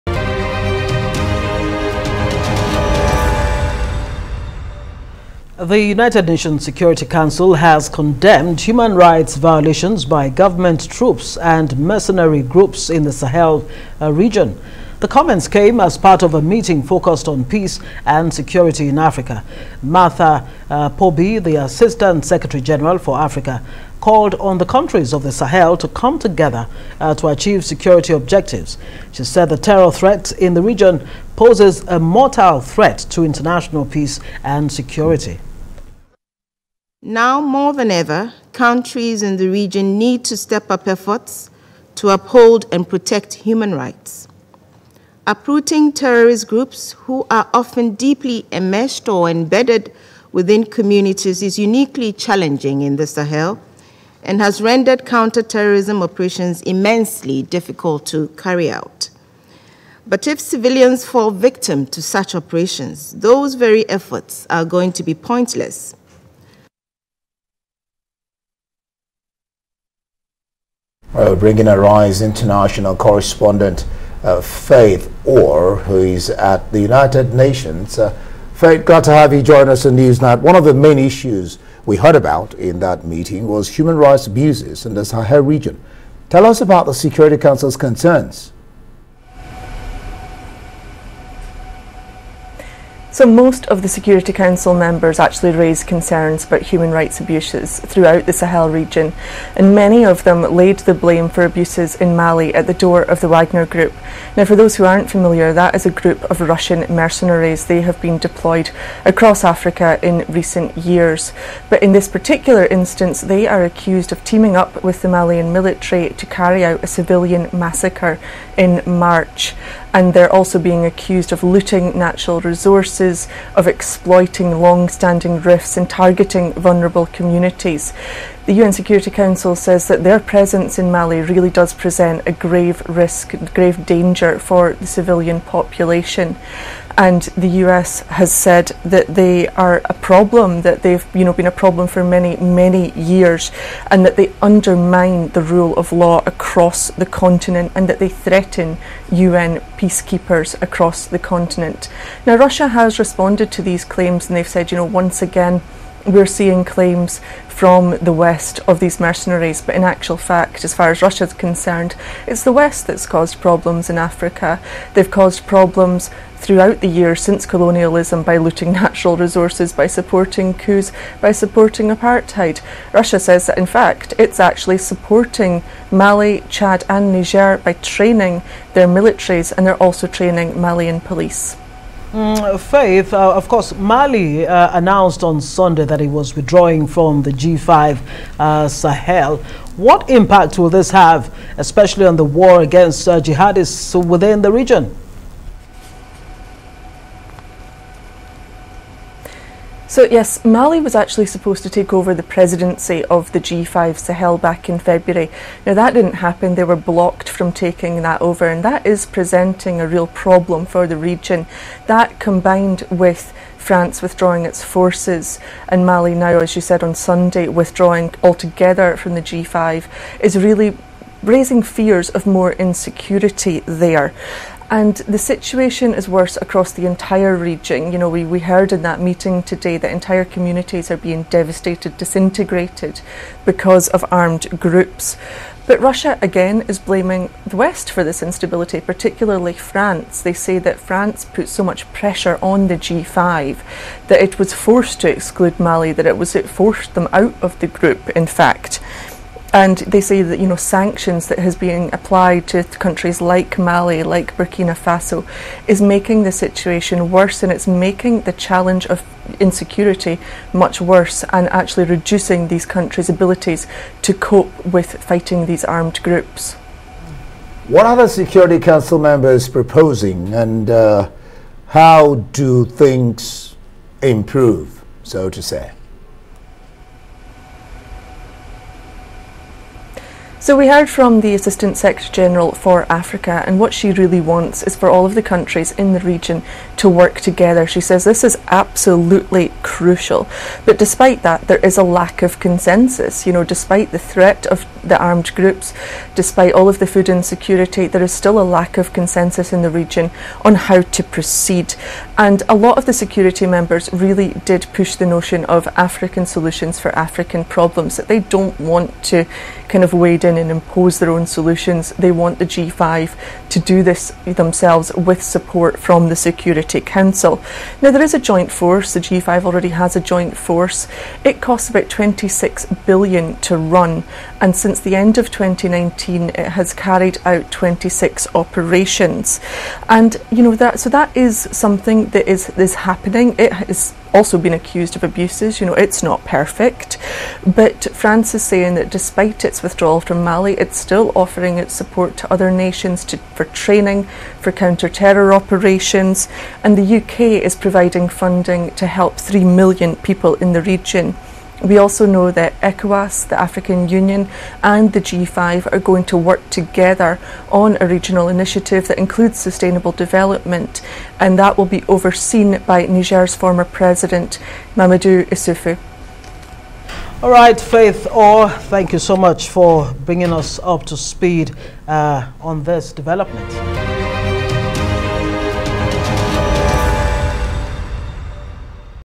Russian Mercenaries Accused Of Colluding With Malian Military Over Civilian Massacre - Faith Orr
The United Nations Security Council has condemned Human Rights violations by governments and mercenaries in the Sahel region the comments came as part of a meeting focused on peace and security in Africa. ARISE International Correspondent, Faith Orr report from the UN office an the allegations of Russian mercenaries involvements in extrajudicial killings in the Sahel.